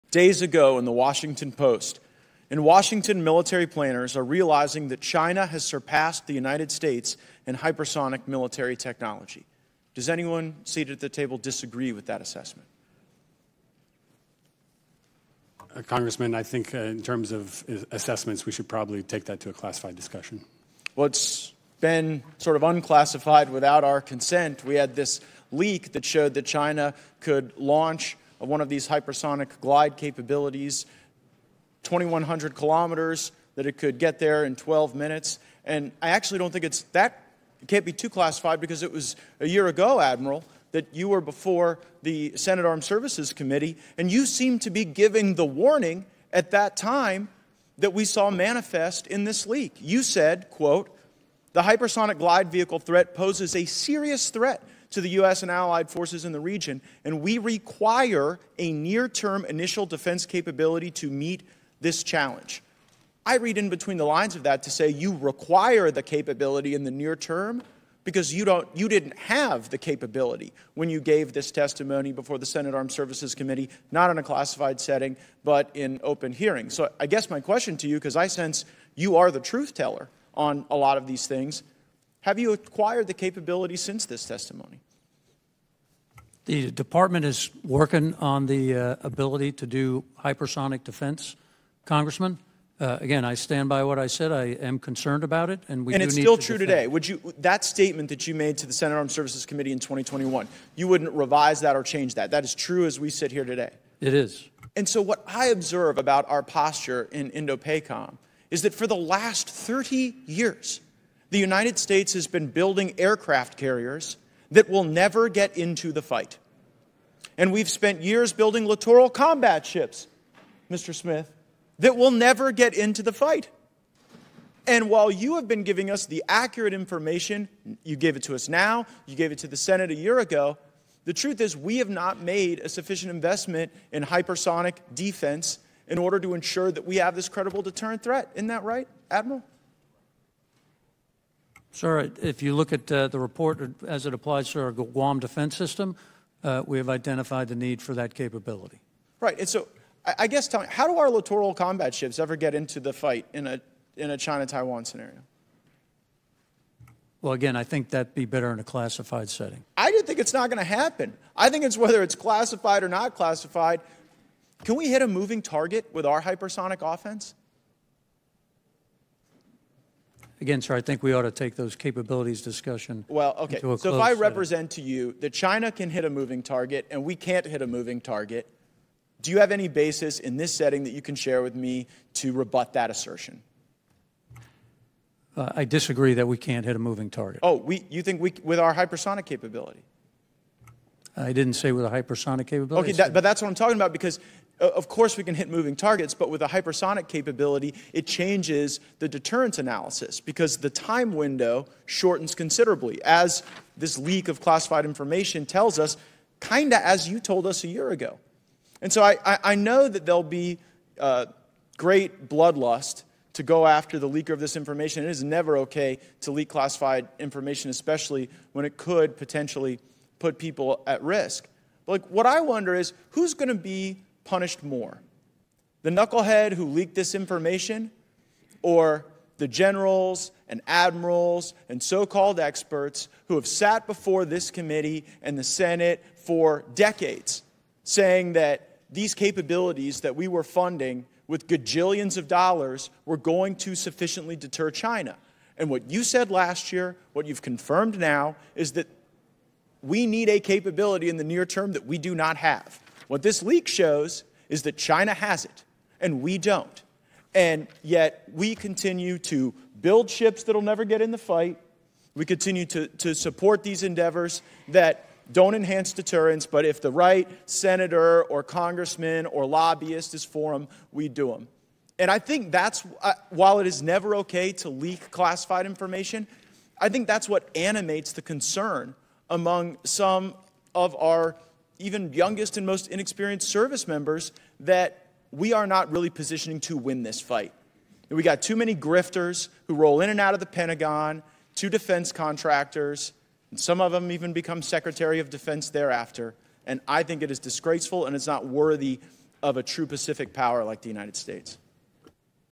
Grifters at the Pentagon LIED About U.S. Hypersonic Capabilities!
The head of the U.S. Indo-Pacific Command just reiterated the same concerns he had last year: America needs hypersonic capabilities in the near term that we currently do not have. Meanwhile, China is surpassing our hypersonic capabilities.

So who’s going to be punished more: the knucklehead who leaked the classified information confirming this or the Generals, Admirals, and so-called “experts” who sat before our committee for decades and lied?

We have too many grifters who roll in and out of the Pentagon to defense contractors – some of them even become Secretary of Defense thereafter.

(U.S. Military Posture and Nat. Sec. Challenges in the Indo-Pacific Region, House Armed Services, 04/18/2023)

for ALL Firebrand content!